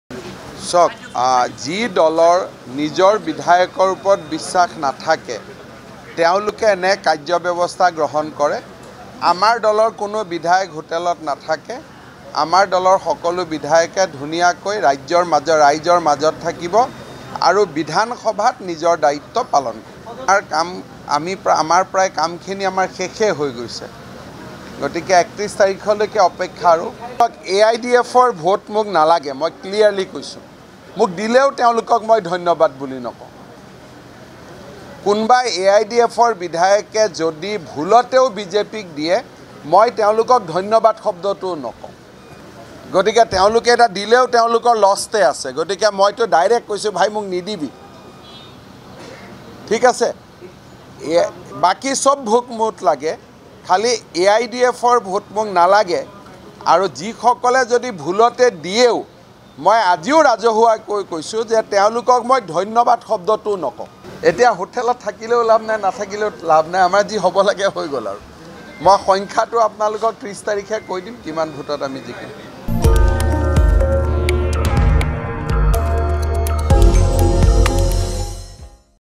ভুলতে ভোট দিলেও মই ধন্যবাদ বুলিও নক‌ও AIUDFক :মুখ্যমন্ত্ৰী -News Network assam
যি দলৰ নিজৰ বিধায়কৰ উপস্থিত বিশ্বাস নাই সেই দলে বিধায়ক হোটেলত ৰাখিব ।BJP ৰ বিধায়ক সকল ৰাইজৰ মাজত থাকিব।ৰিপুন বৰাই মিঠাই খোৱাইছে ভাল কথা।মোক মিঠাই দিলে ম‌ইও খাম।AIUDF ৰ ভোট মোক নালাগে।ভুলতে ভোট দিলেও মই ধন্যবাদ বুলিও নক‌ও AIUDF ক ।উজনি অসমৰ বুদ্ধিজীৱীয়ে আজমলে অসমৰ নেতৃত্বত লোৱাৰ সন্দৰ্ভত মই একো নক‌ও ।কিন্তু ৰাজহুৱাকৈ কৈছো AIUDF য়ে ভোট দিলেও ধন্যবাদ নিদিও।ৰাজ্যসভাৰ দুয়োখন আসনত আমি জয় হ'ব ।আমাৰ কাম হৈ গৈছে , কংগ্ৰেছৰ বিধায়ক হোটেলত ৰাখিব কাম নাই।৩০ তাৰিখে আমি কিমান ভোট পাম মই কম:মুখ্যমন্ত্ৰী।